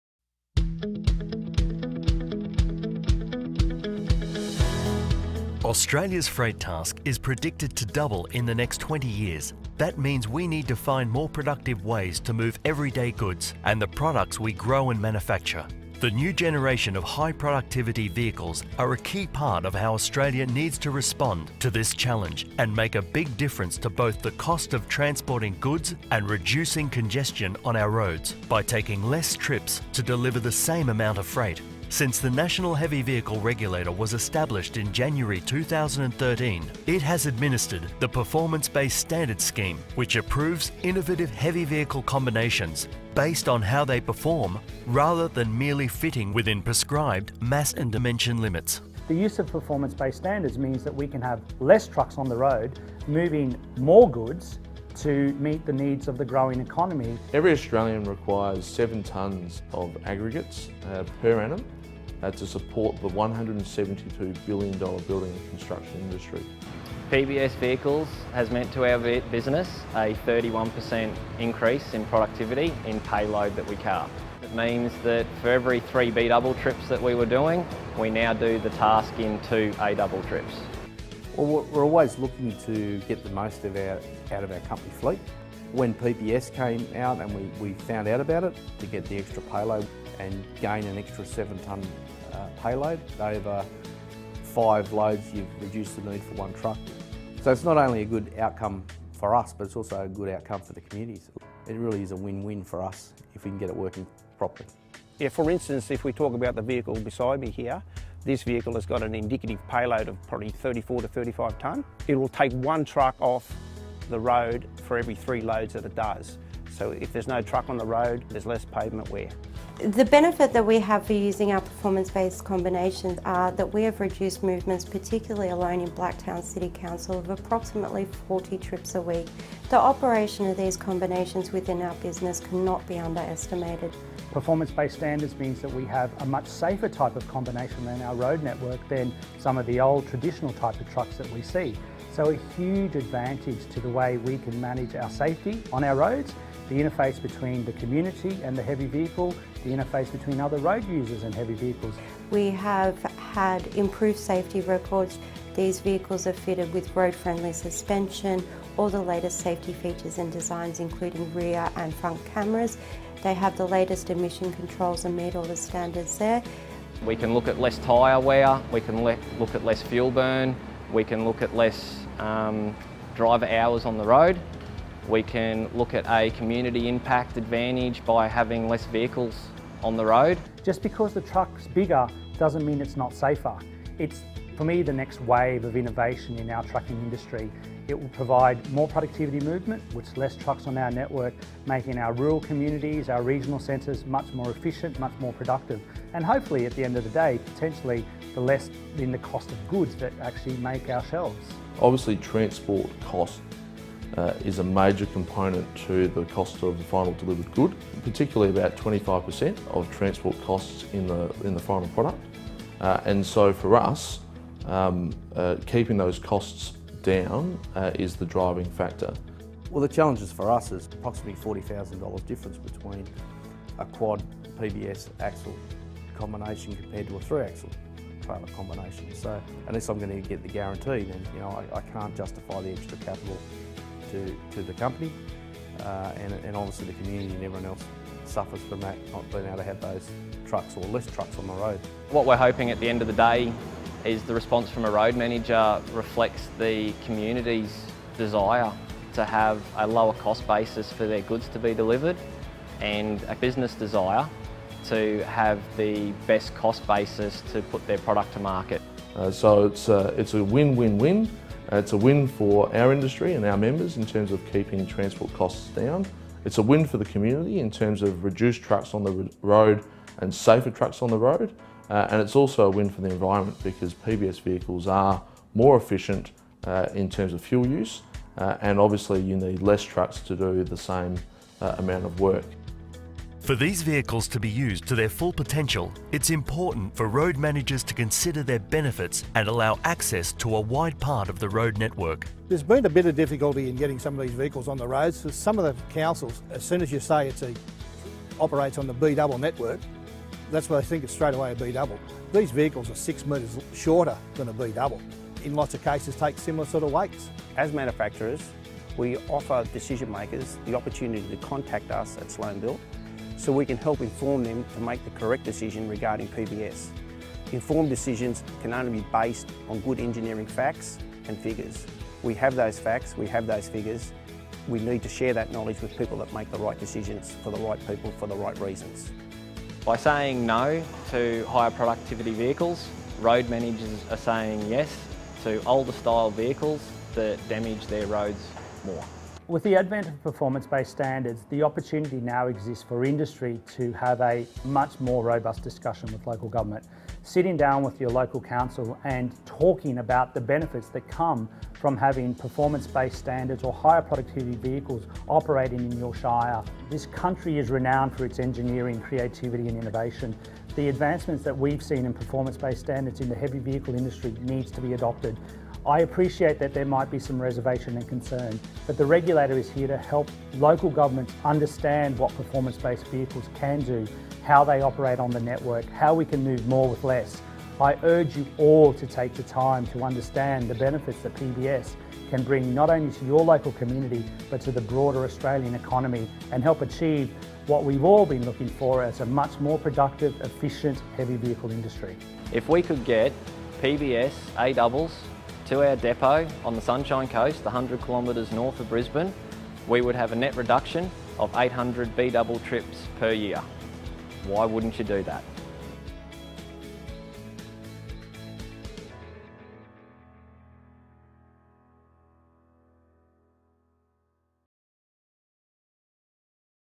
PBS NHVR- The Big Picture feat Sloanebuilt|| Australian Road Safety
The NHVR has launched a new video to further assist local government to continue to improve the productivity and safety of the heavy vehicle industry.